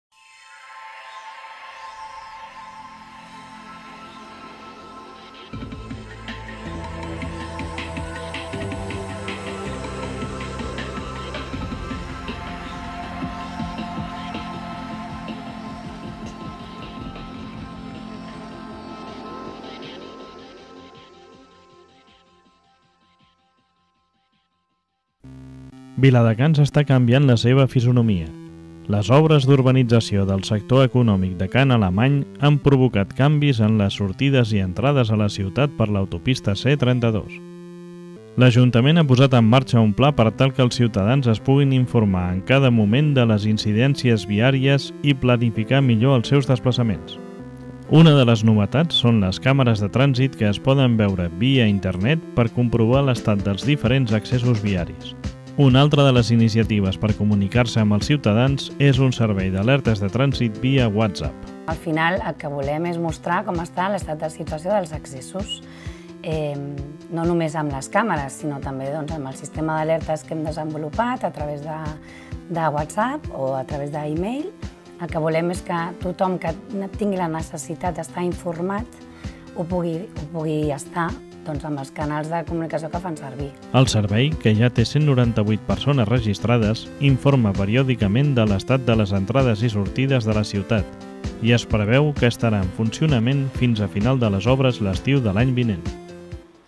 Com estar al dia sobre el trànsit a Viladecans.
Viladecans està canviant la seva fisonomia. Les obres d’urbanització del sector econòmic de Ca n’Alemany han provocat canvis en les sortides i entrades a la ciutat per l’autopista C-32.

L’ajuntament ha posat en marxa un pla per tal que els ciutadans es puguin informar en cada moment de les incidències viàries i planificar millor els seus desplaçaments.

Una de les novetats són les càmeres de trànsit que es poden veure via internet per comprovar l’estat dels diferents accessos viaris.

Una altra de les iniciatives per comunicar-se amb els ciutadans és un servei d’alertes de trànsit via WhatsApp

El servei, que ja te 198 persones registrades, informa periòdicament de l’estat de les entrades i sortides de la ciutat i es preveu que estarà en funcionament fins al final de les obres l’estiu de l’any vinent.